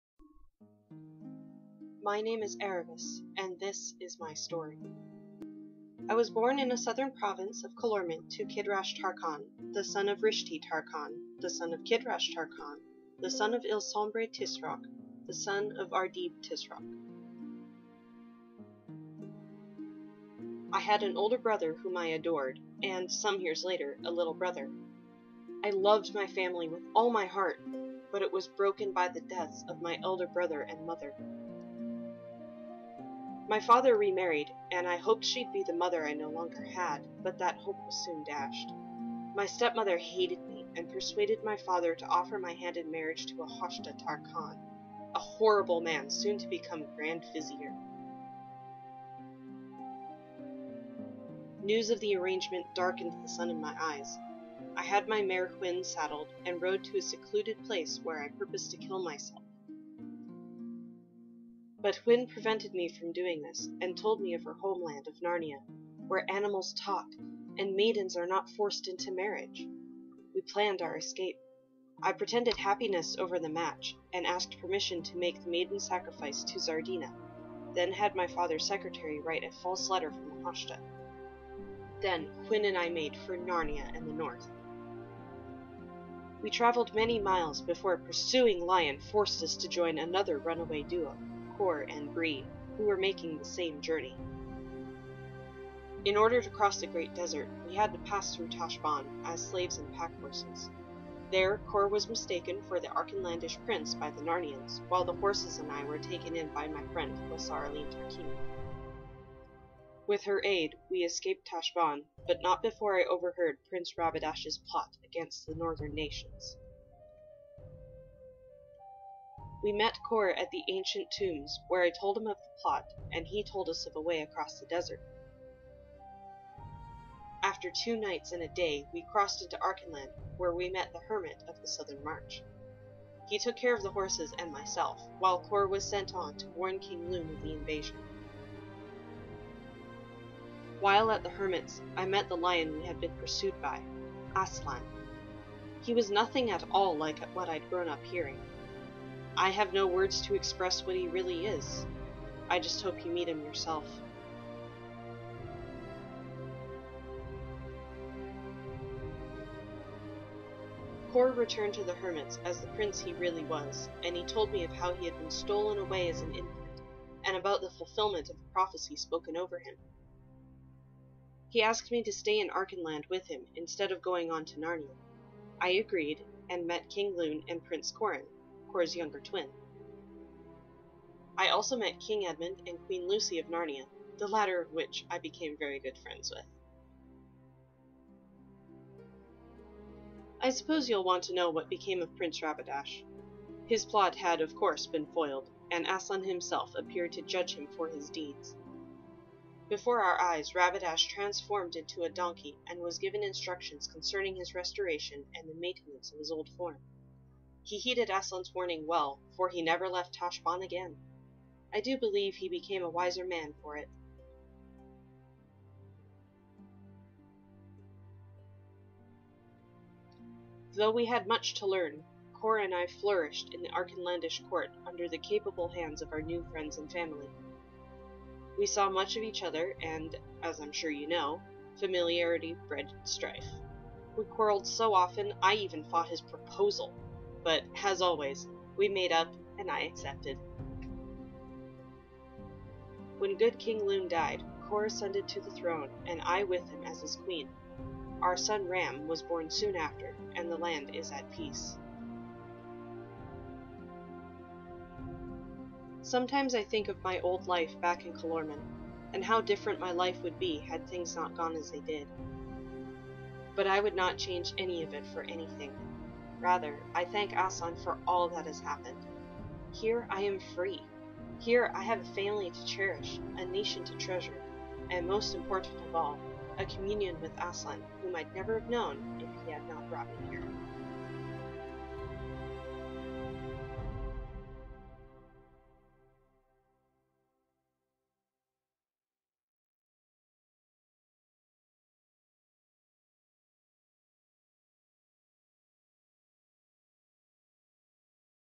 Draw My Life - Aravis Tarkheena
Draw My Life videos, where a person tells their life story and illustrates it on a white board, are a popular trend on youtube.  Even Aravis of Calormen decided to make one.